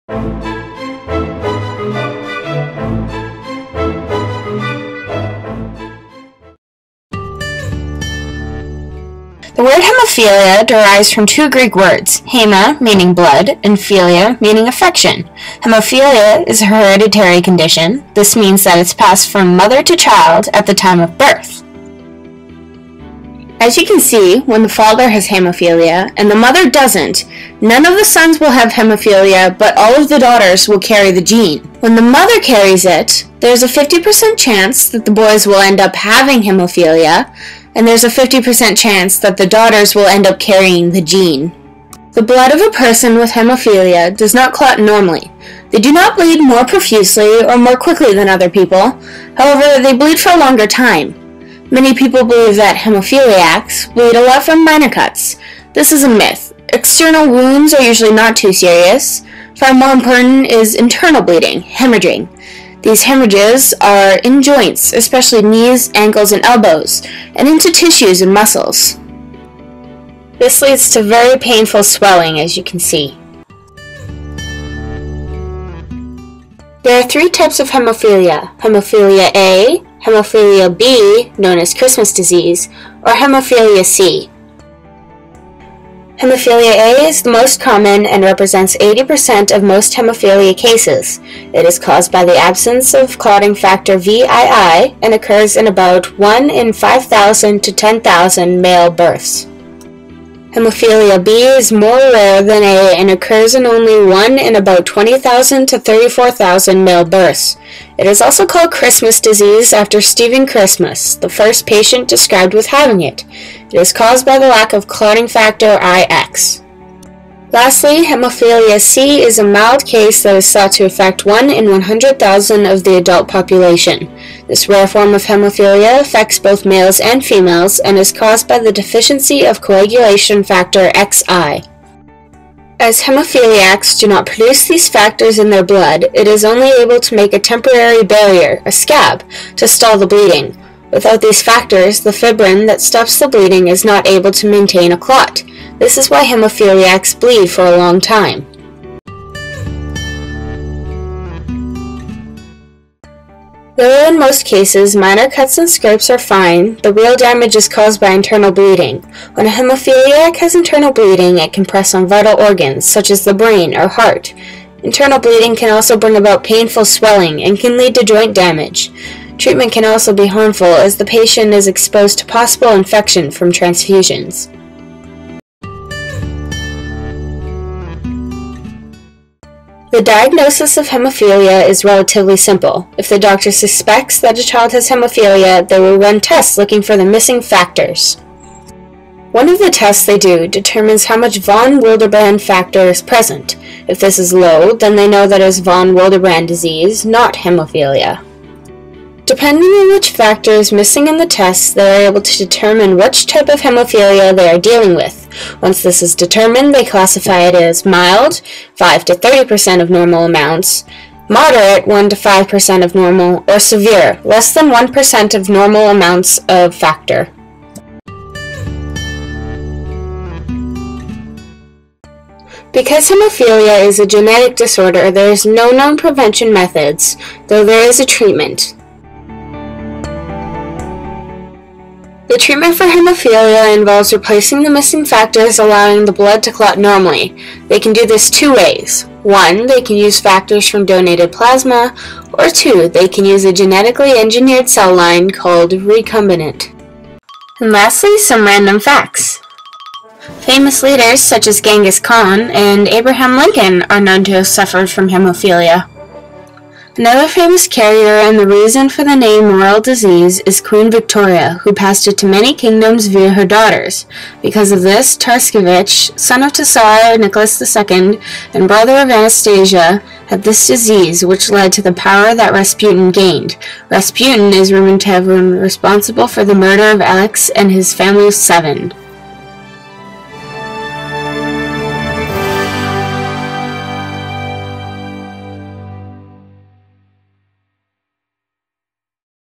Hemophilia The Royal Disease
My Bio 20 project on Hemophilia for Ms. Rein. Hope you enjoy.

Thanks!
Bye~